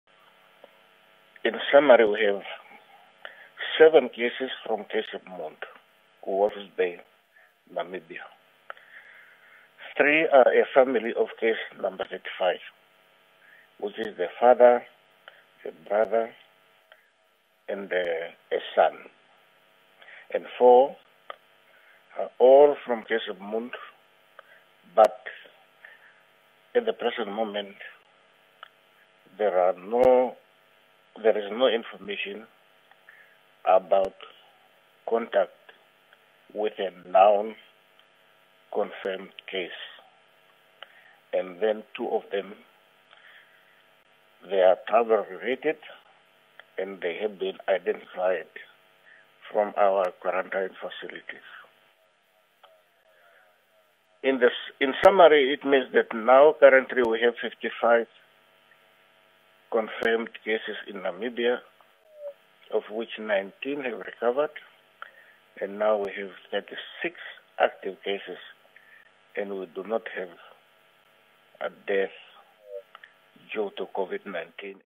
NAMPA TV: Namibia recorded nine new COVID-19 cases in the past 24 hours 21 June 2020
WINDHOEK, 21 JUN (NAMPA) -Minister of Health and Social Services, Dr Kalumbi Shangula announced nine new COVID-19 cases in the past 24 hours bringing the total number of confirmed cases to 55. He was speaking at the COVID-19 Communication Centre here on Sunday. (Video by Lydia Pitiri) NAMPA